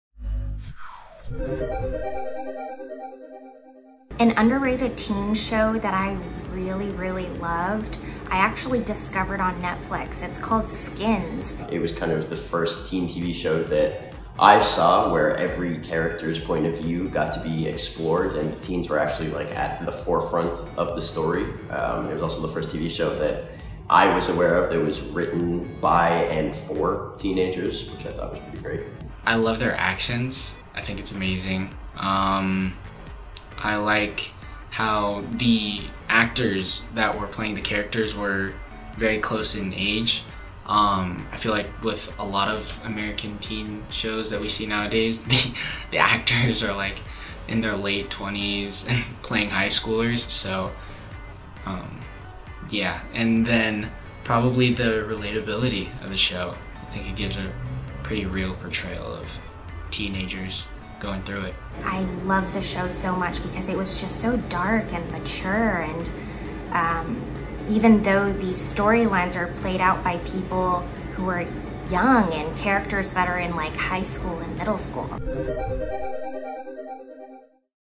Ramona Young, Dallas Liu and Uly Schlesinger Love 'Skins' | Celebrating Art | Entertainment Weekly
Teen TV stars Ramona Young, Dallas Liu and Uly Schlesinger discuss why they love the British show 'Skins' for EW's Celebrating Art series.